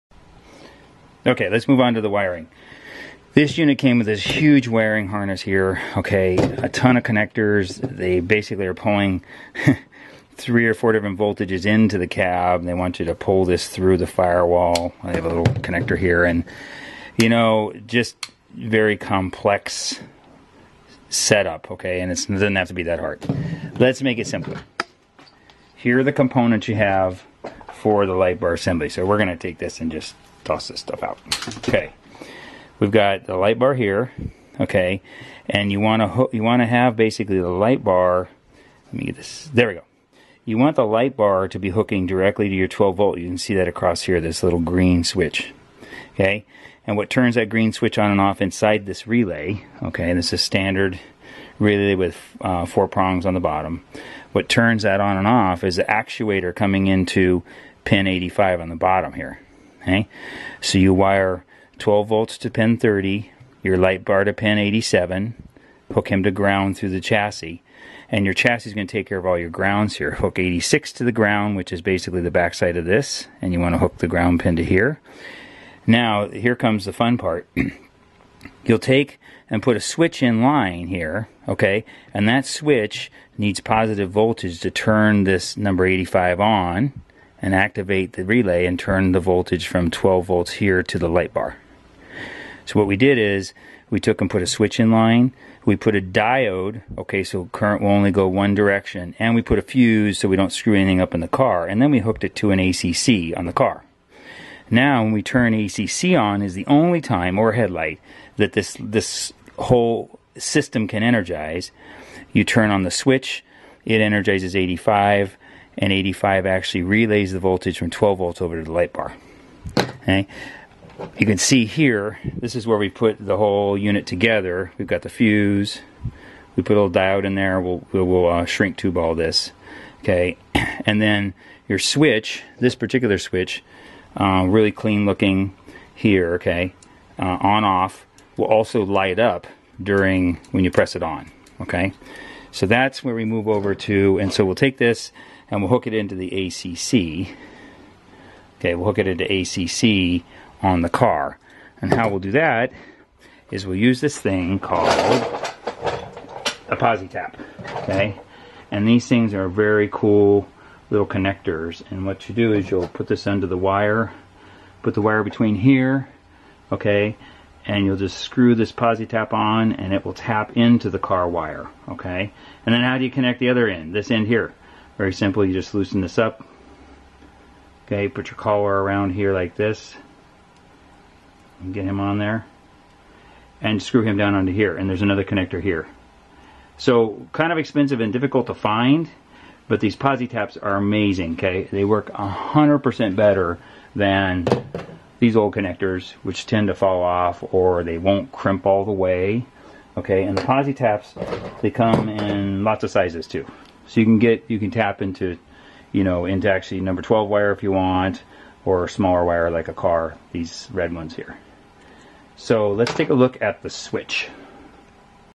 Wiring LED LIght Bar Part 1, Relay and switch GMC colorado
part 1:  How the relay works to "latch" the ACC and turn on the relay, and light.